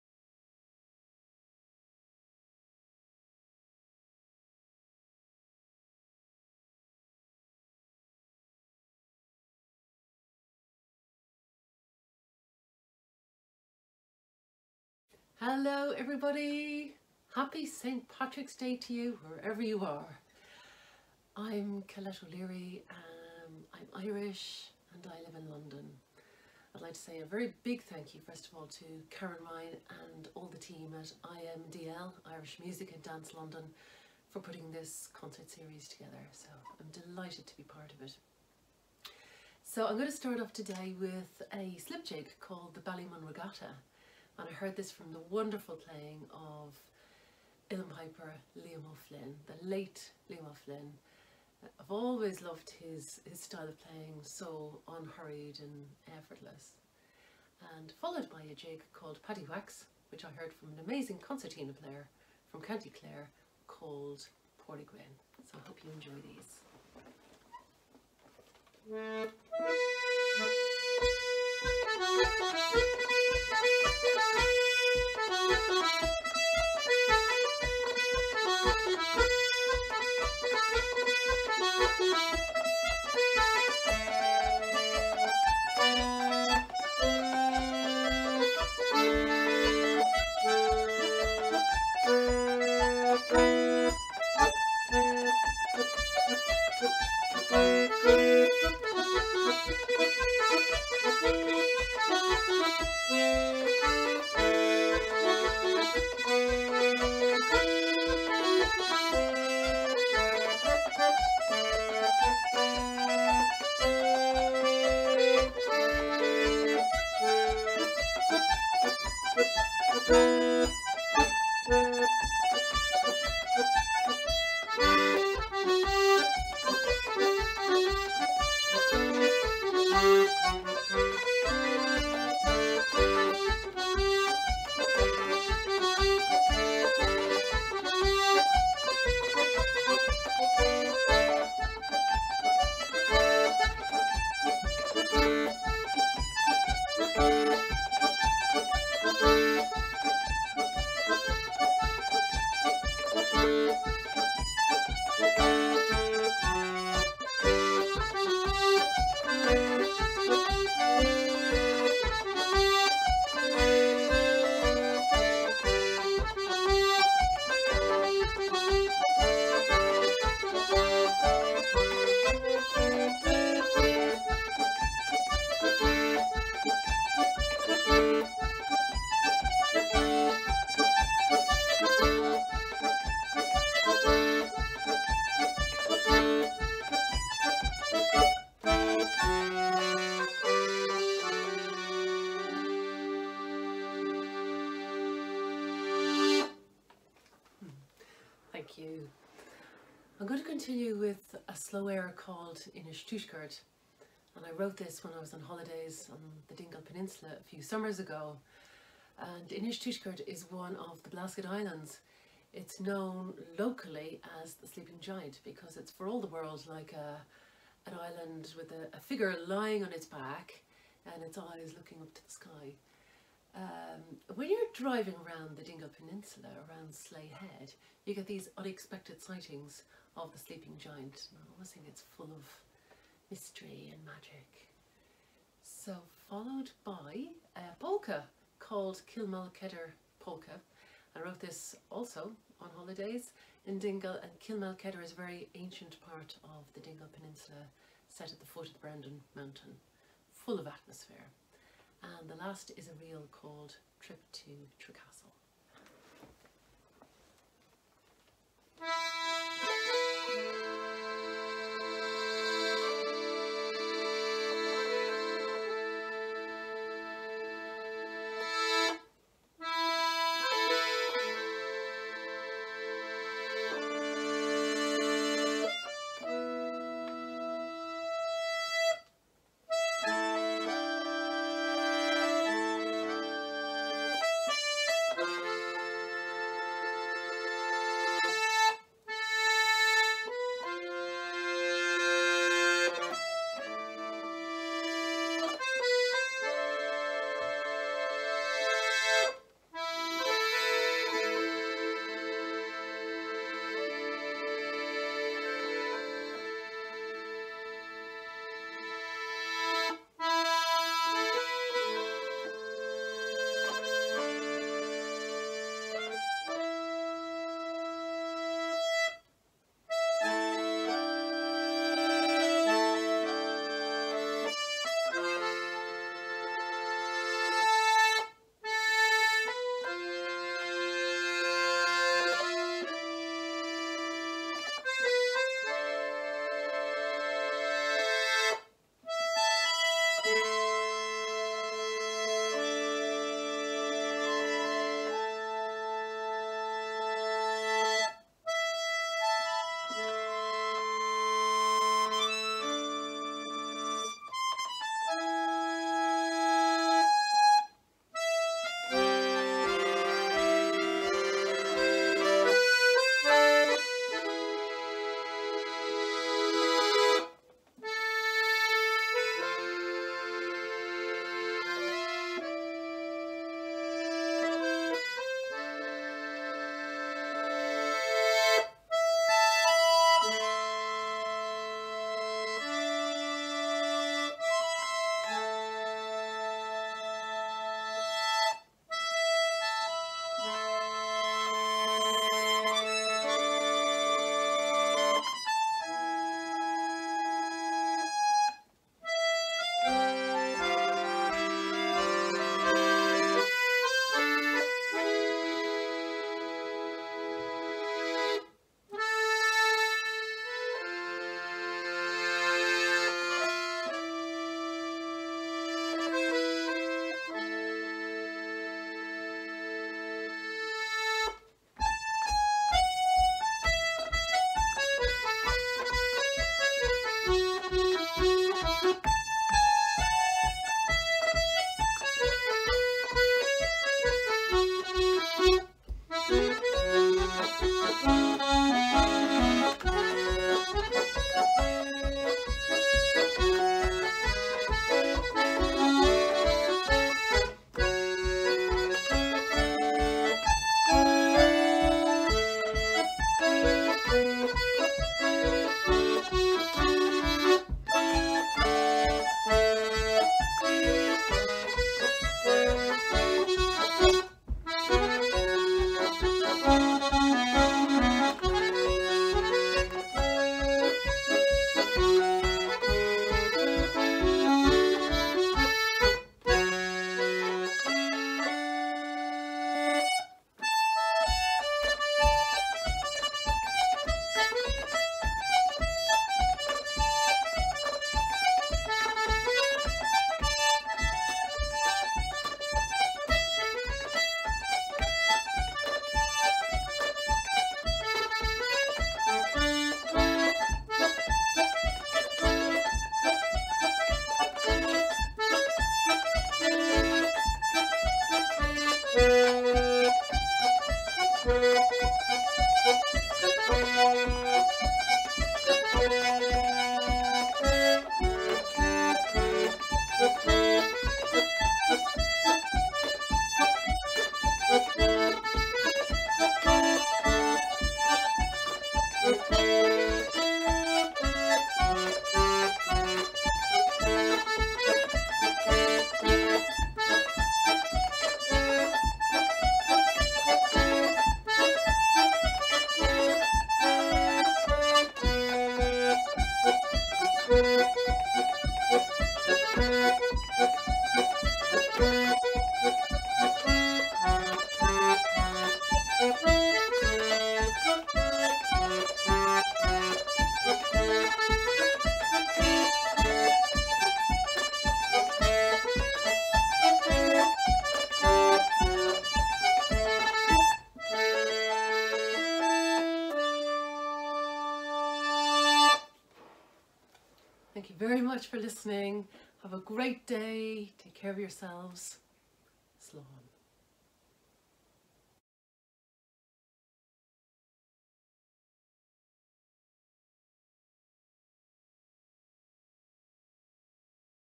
Colette O'Leary (piano accordion)- IMDL 'Solos at Home' mini concert series - St Patrick's Week 2021
Irish Music and Dance in London present:
Colette O'Leary (piano accordion)
as part of the IMDL 2021 Digital Concert Series -
'Solos at Home'
St Patrick's Week 2021

Thanks to the Irish Embassy in London for their support in this mini concert series CreativeCommunity​